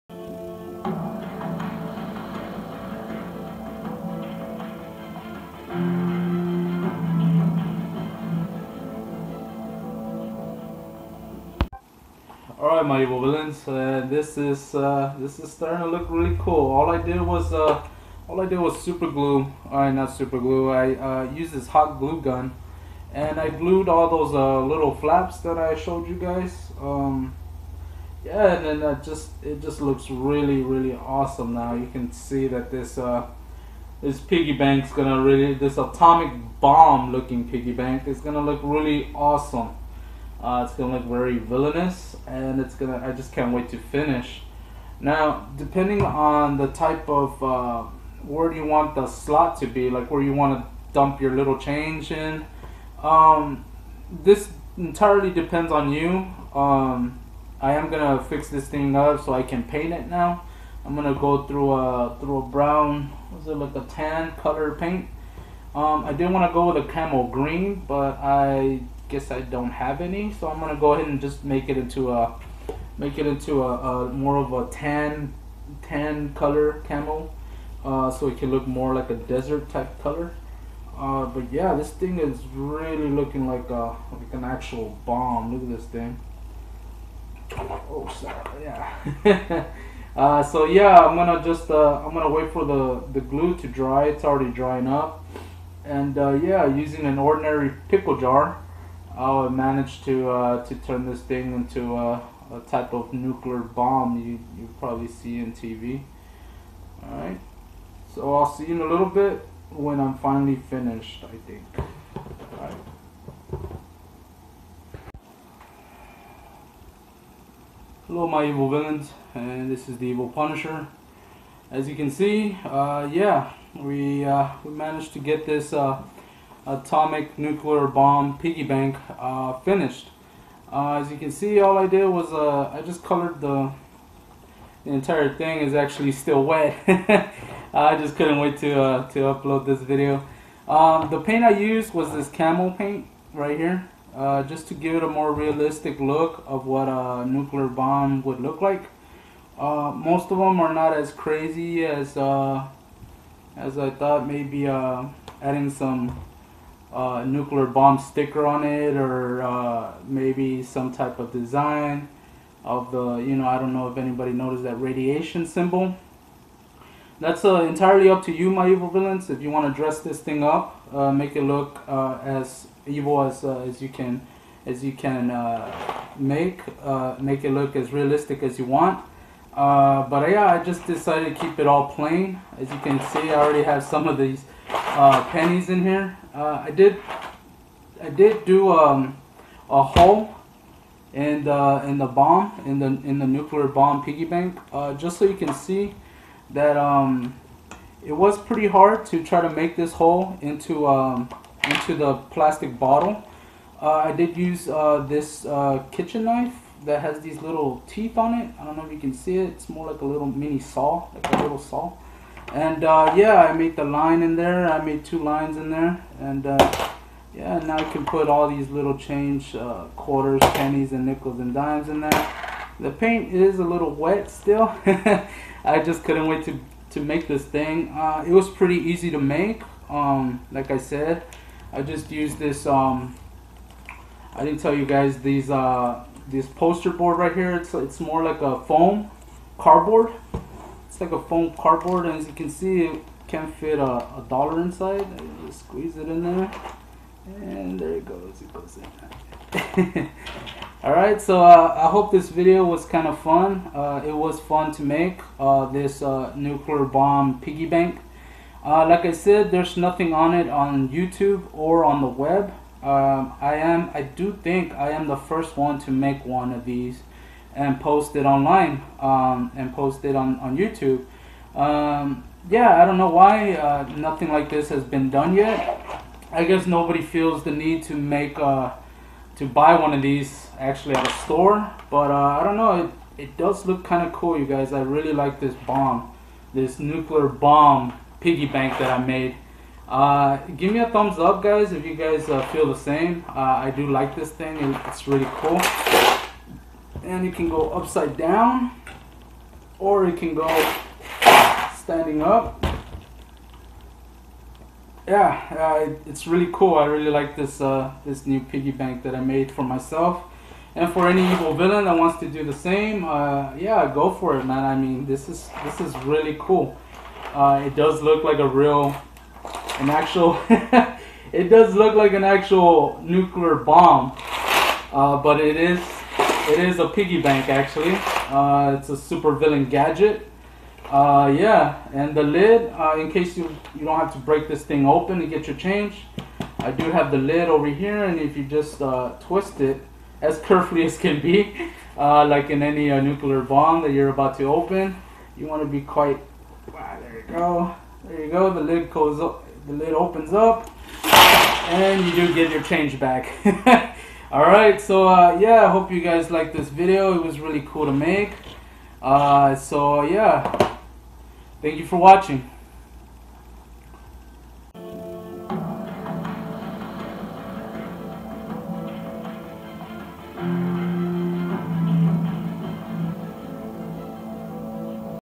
LET'S BUILD A NUCLEAR BOMB!!! D.I.Y "coin bank" SUPER VILLAIN gadget
The very first NUCLEAR BOMB "PIGGY BANK" ever invented was by a REAL LIFE SUPER VILLAIN!!!👏👏👏👏👏👏 HAHAHA!!! I decided to MAKE my own because there is nothing like it made in the WORLD!!!💣💣🔥🔥🔥🔥😁😁😁😁